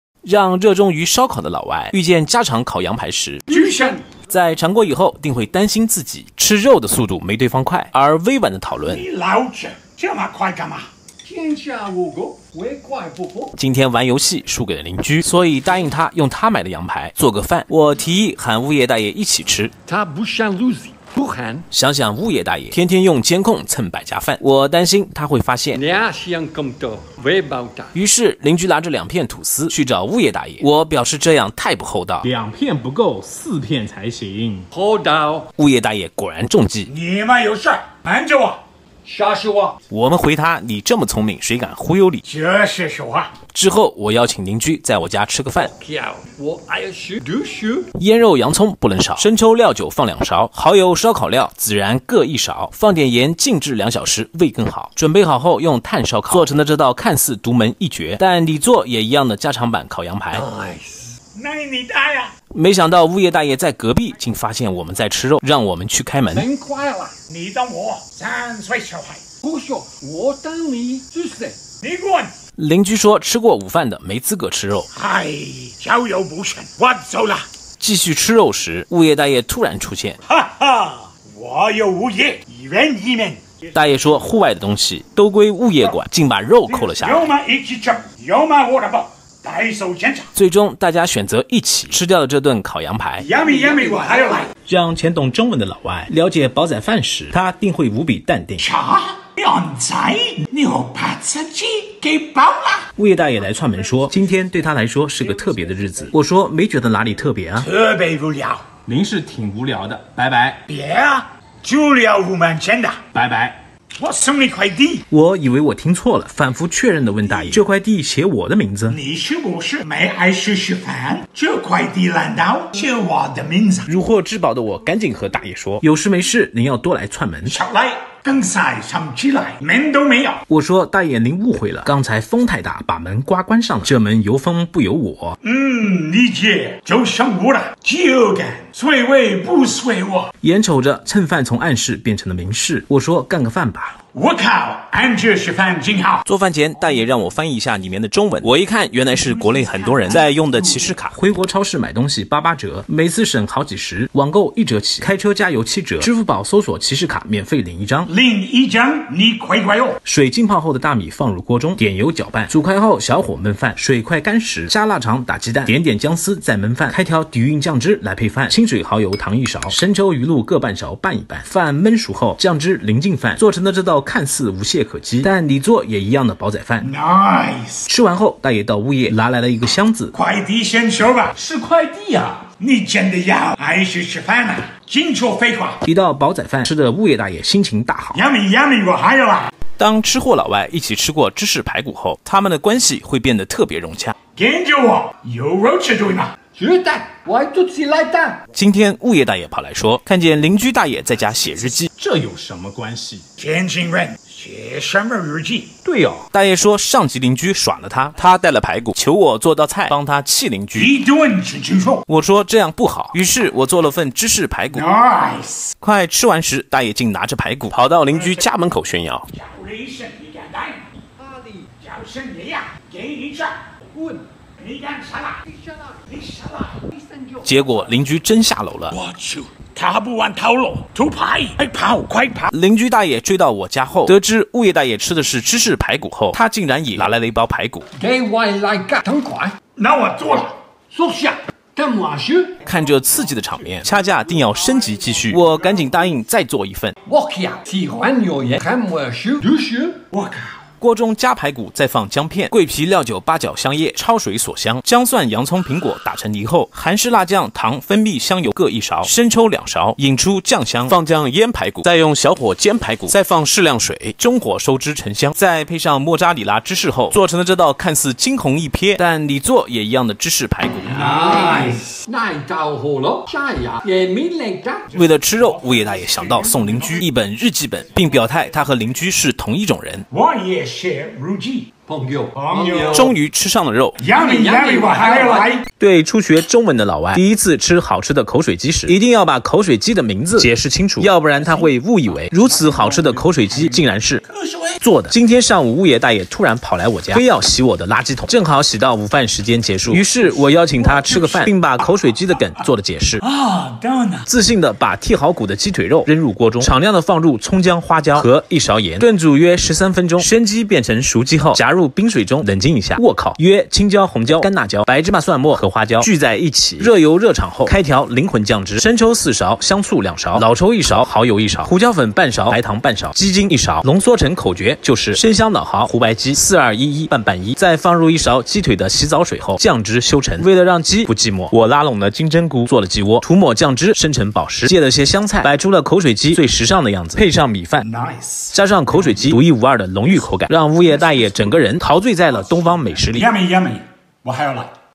澳洲大表哥vlog合集（13）- 在尝试过美食以后，会担心自己吃肉的速度没对方快～
大纲内容：-
抖音账号：Oz_big_brother